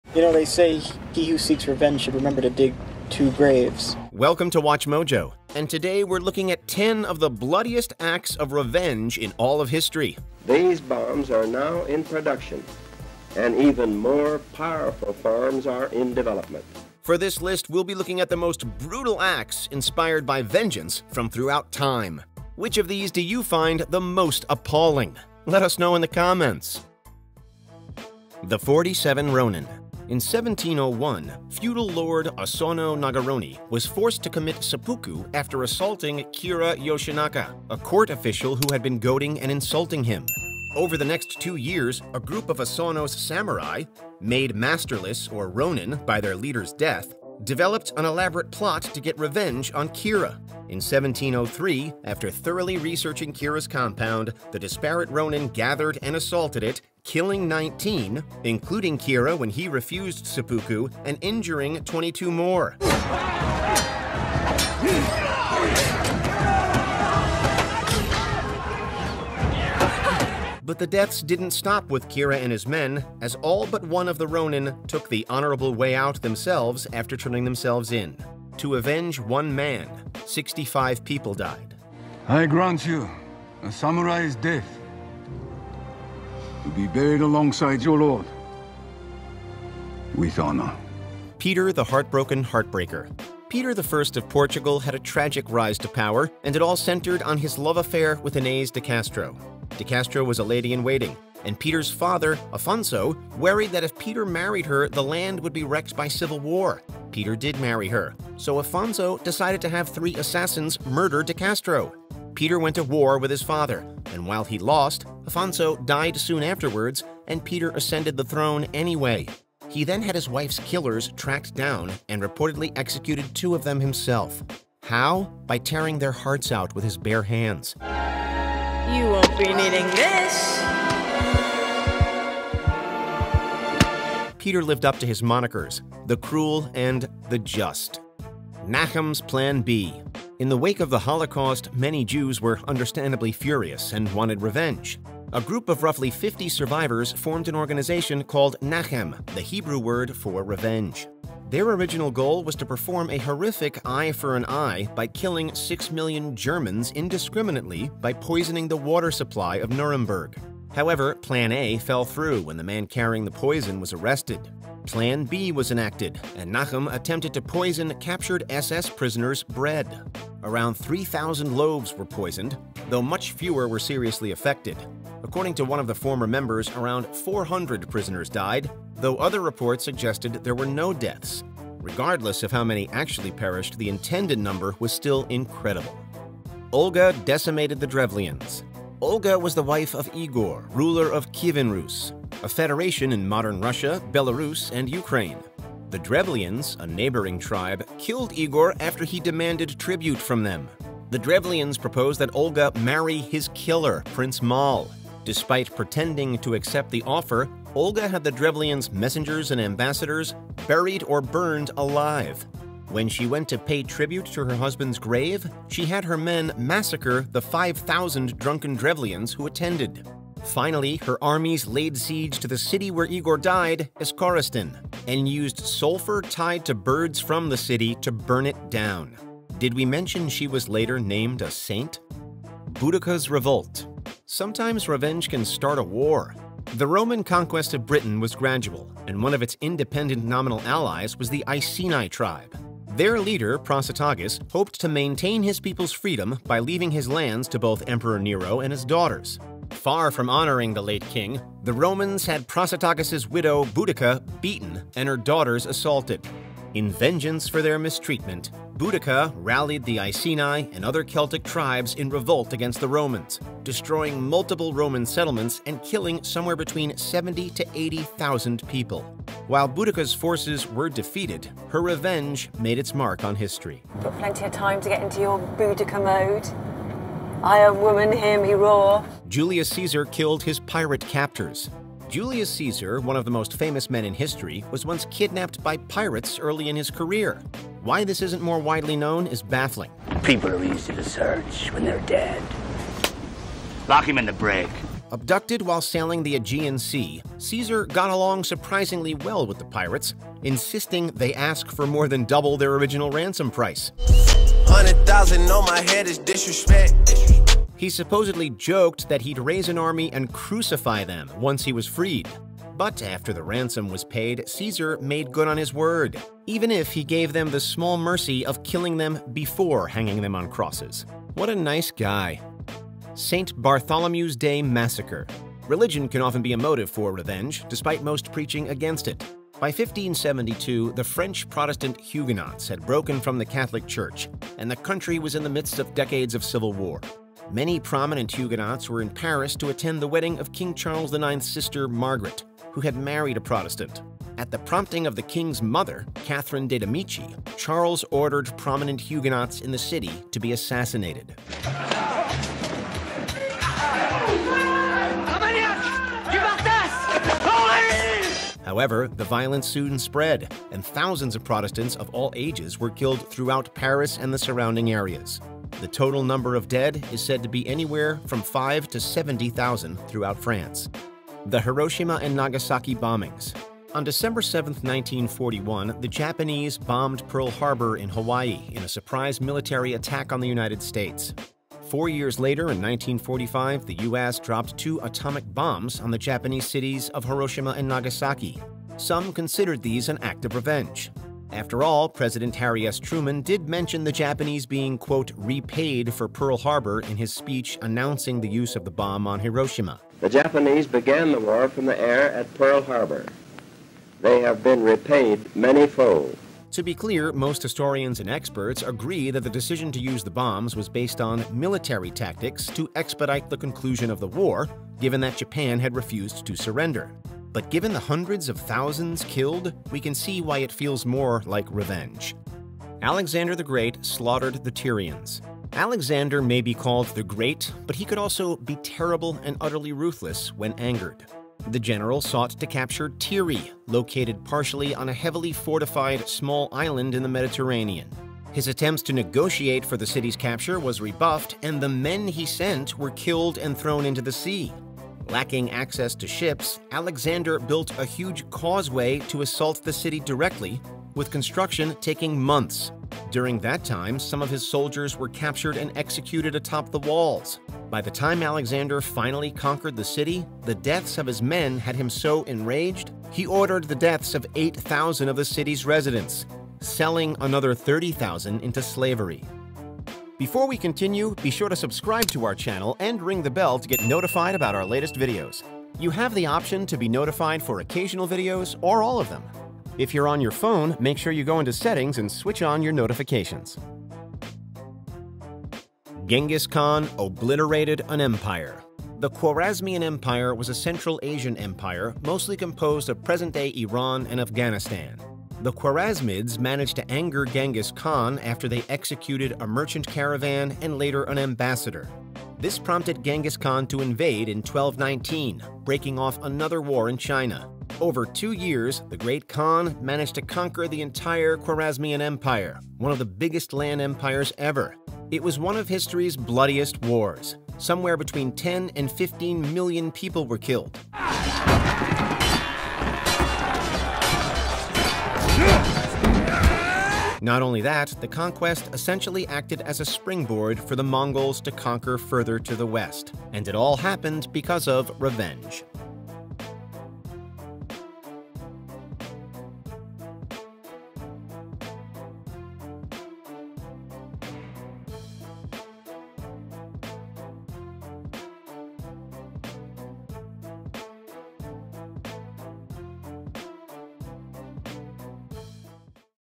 10 Bloodiest Acts of Revenge In History
History is written in blood. For this list, we’ll be looking at the most brutal acts inspired by vengeance from throughout time. Our countdown includes The Forty-seven Rōnin, Olga Decimated the Drevlians, Julius Caesar Killed His Pirate Captors, and more! Which of these do you find the most appalling?